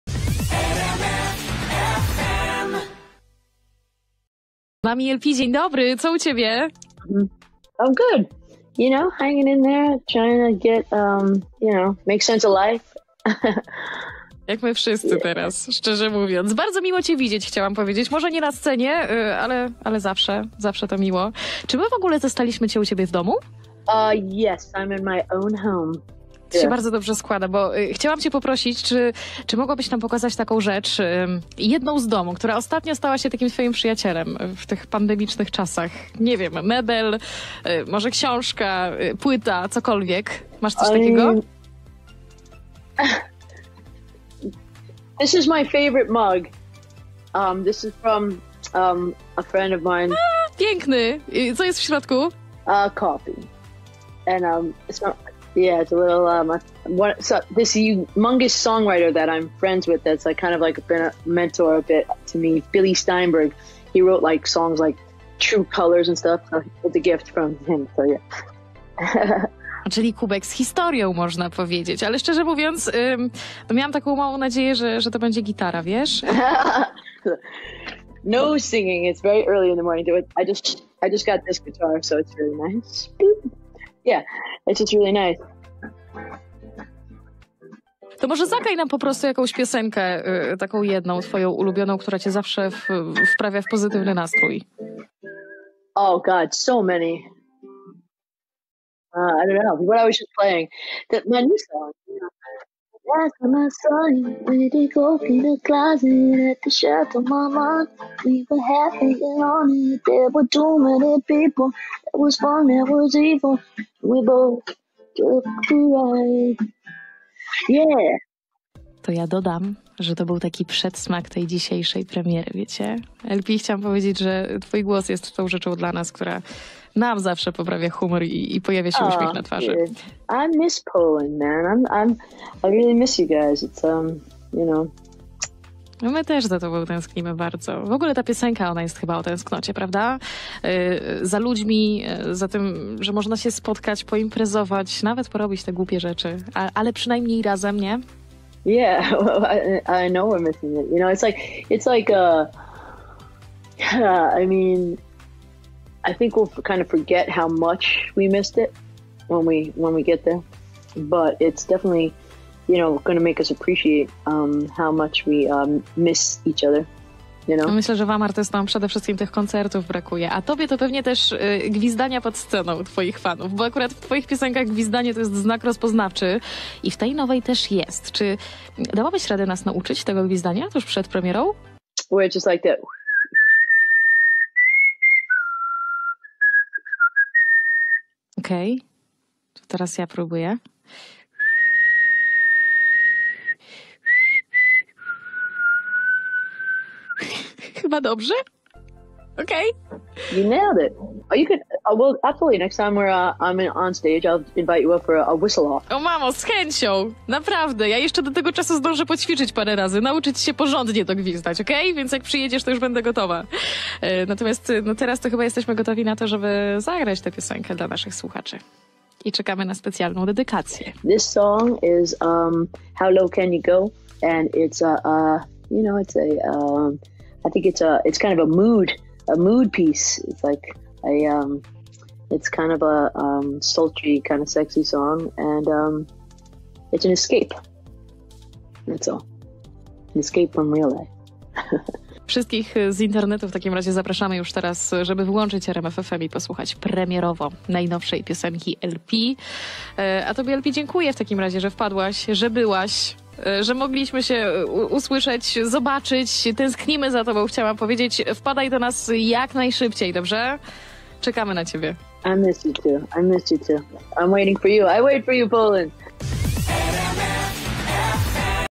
LP interview - for Aleksandra Filipek by RMF FM (traducción: LP Forever Spain) @iamlpofficial
LP  entrevistada por Aleksandra en RMF FM
Traducido y editado por LP Forever Spain